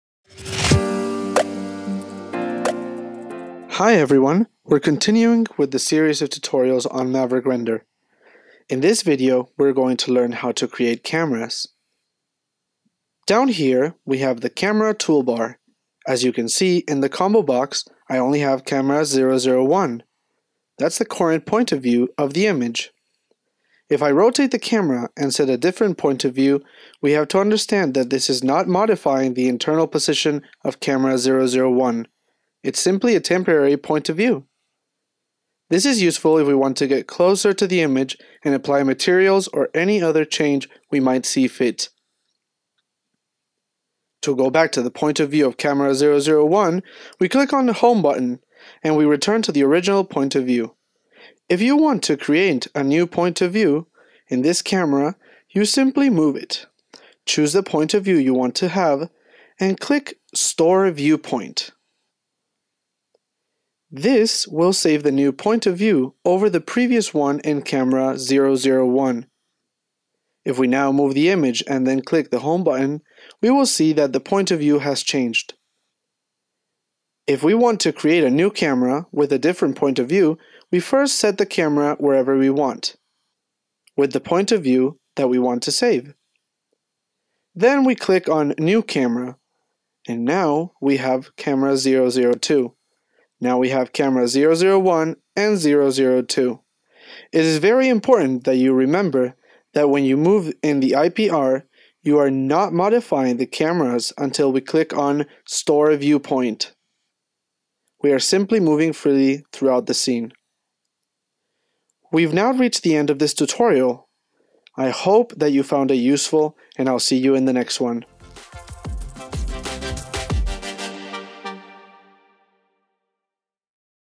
MAVERICK RENDER: 03 CREATE CAMERAS
Learn how you can create cameras in Maverick Render.